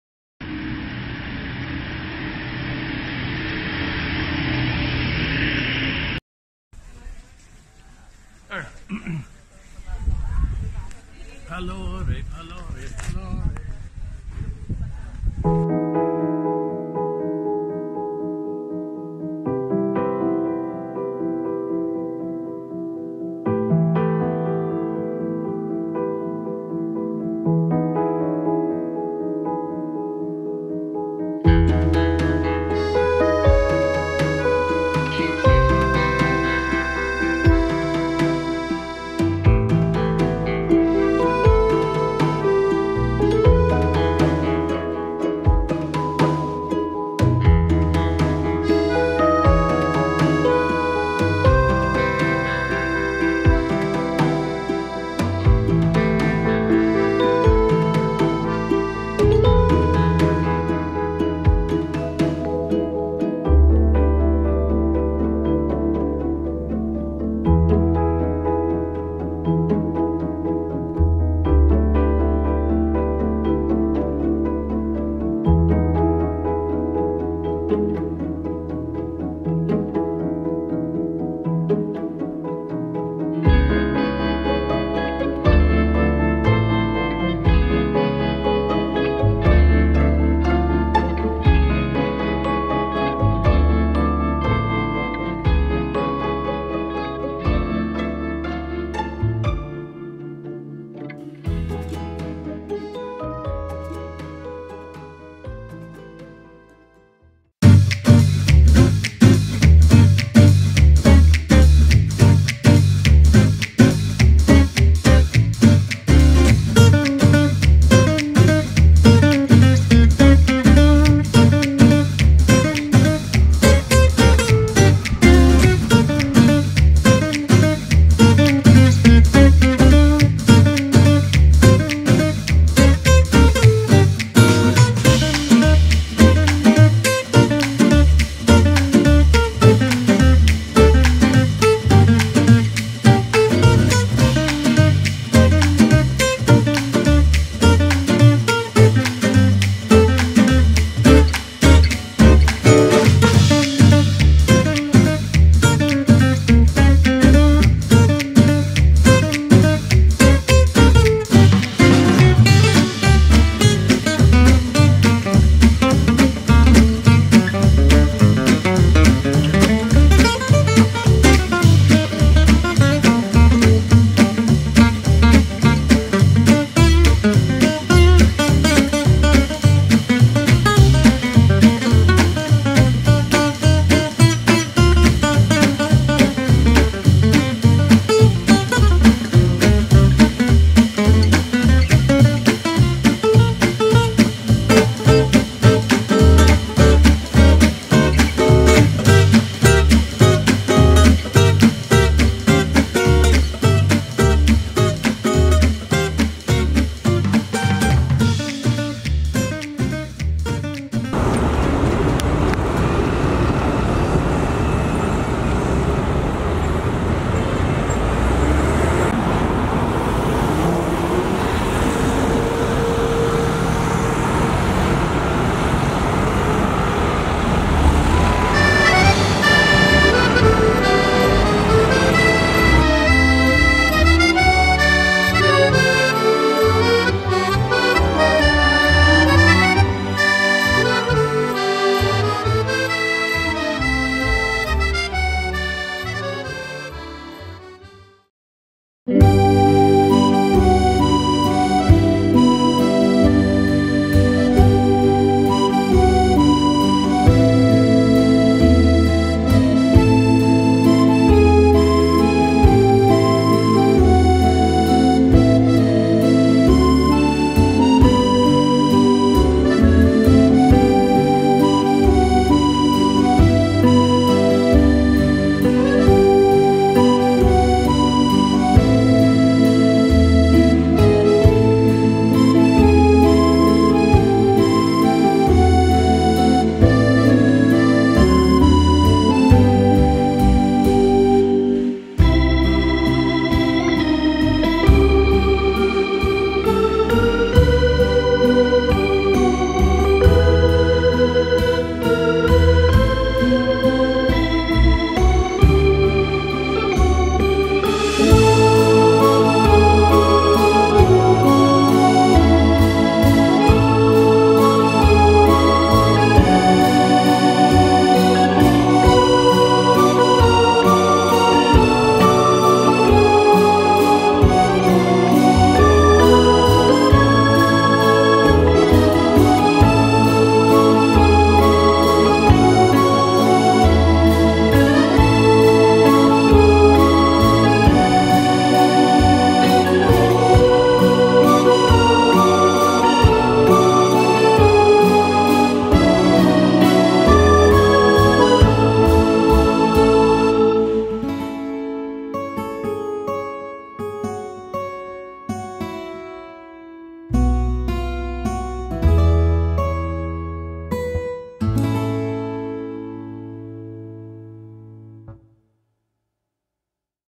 Paris, France Trip 2019 (Part 2)
5 days visit to Paris, France. Considered to be the most visited cities in the world. We saw the charm, character and culture of the city. For the 2nd time visiting it radiates me more to indulge in my curiosity of the city being how it is been visited for all walks of life. I want to share our adventures as we explore Paris.